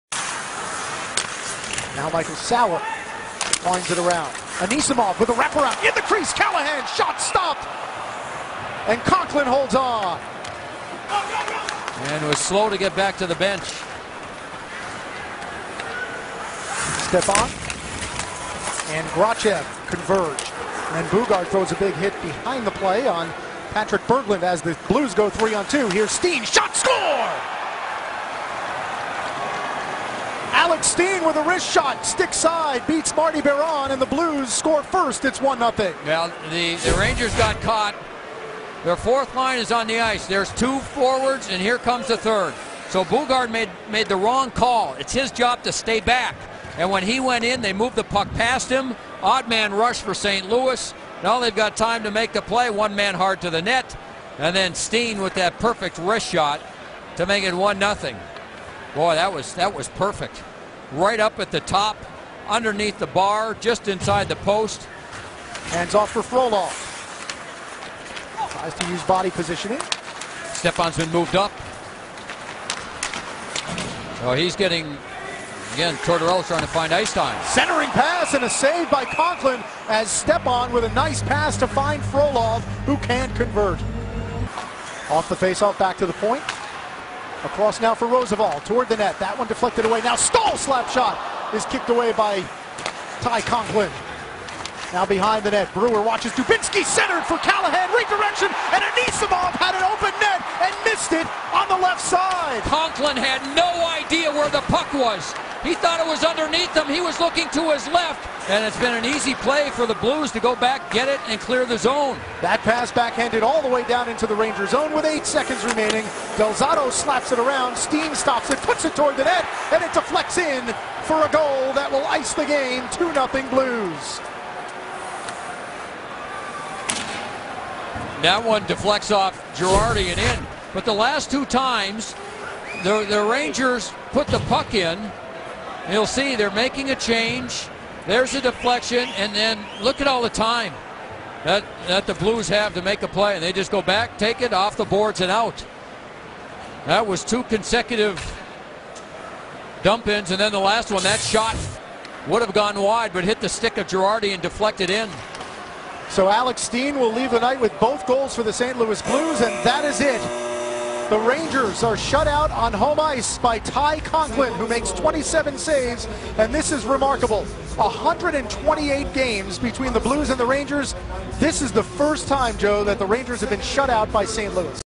Blues @ Rangers 11/7/10
St. Louis Blues @ New York Rangers

2-0 win for St. Louis as back-up goaltender Ty Conklin gets the shutout by making 27 saves for it and the win to exrend the winning streak to 8 games and leading the Western Conference with the L.A Kings with 20 ponts.